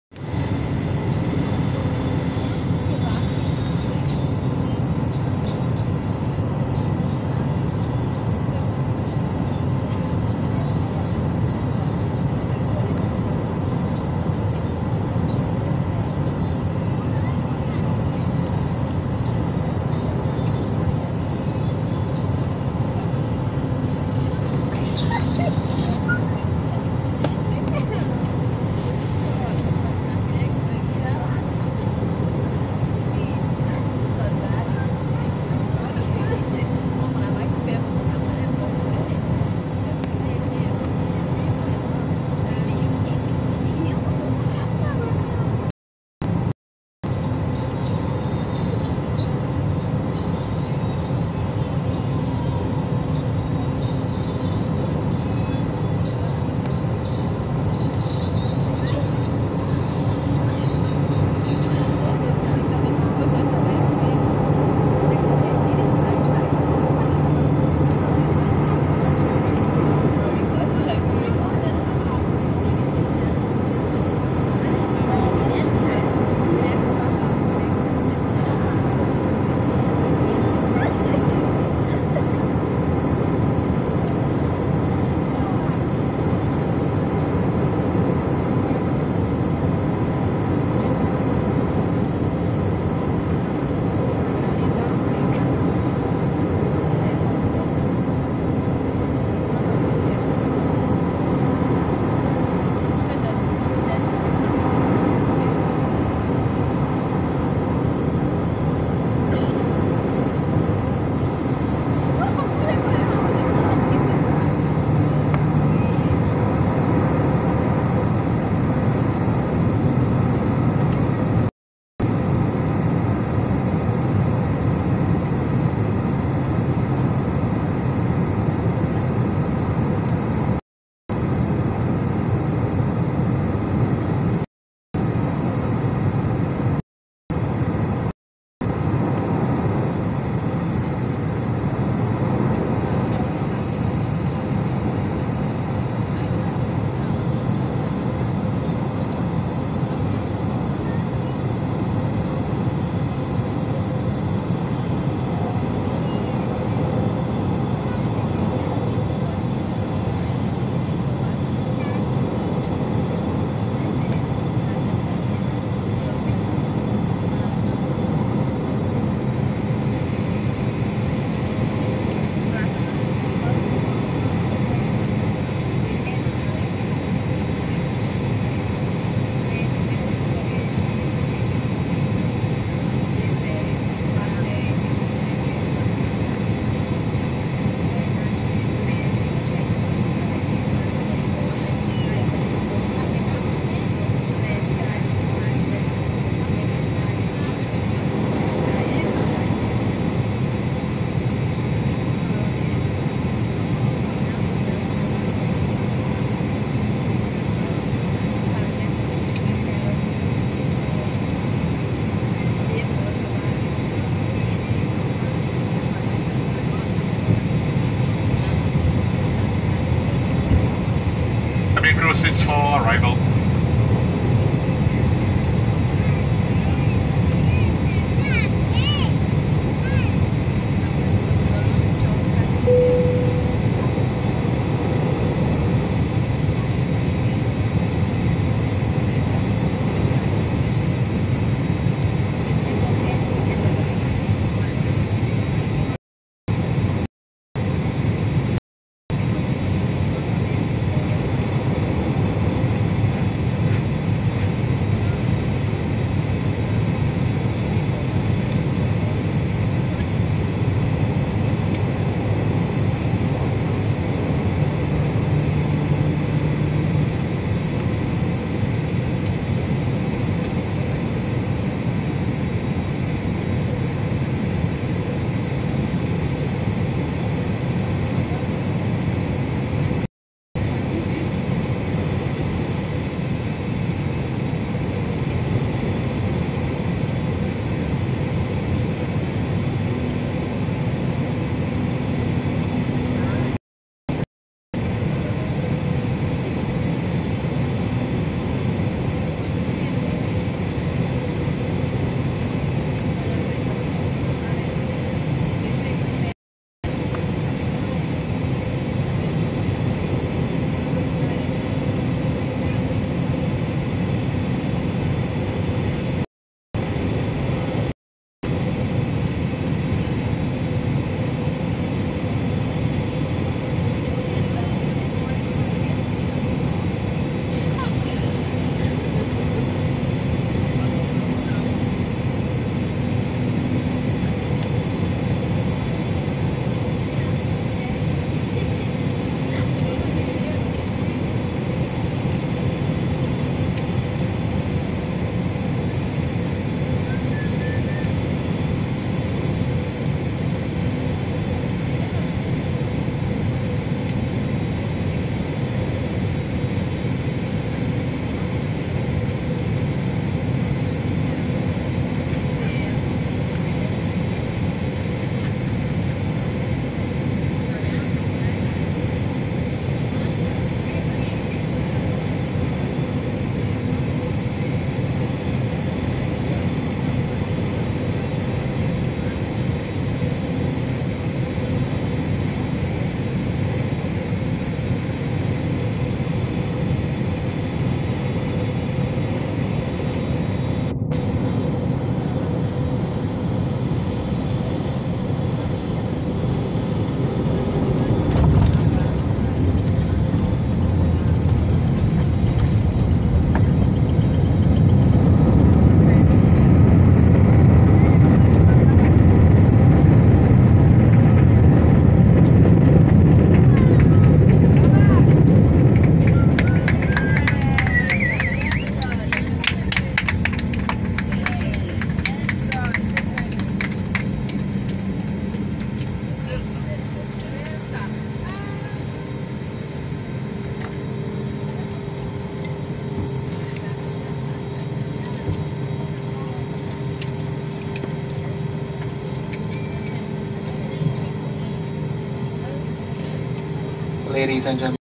Air Seychelles Airbus A320-200nero
HM49 landing in Mauritius on 9th December 2019